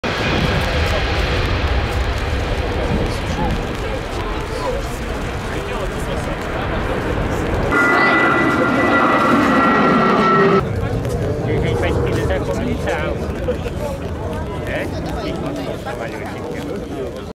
МС-21-300 ARMY 2022 20.08.2022
МС-21 - российский среднемагистральный узкофюзеляжный пассажирский самолёт.
Модификация: МС-21-300
Бортовой номер: 73054 / 003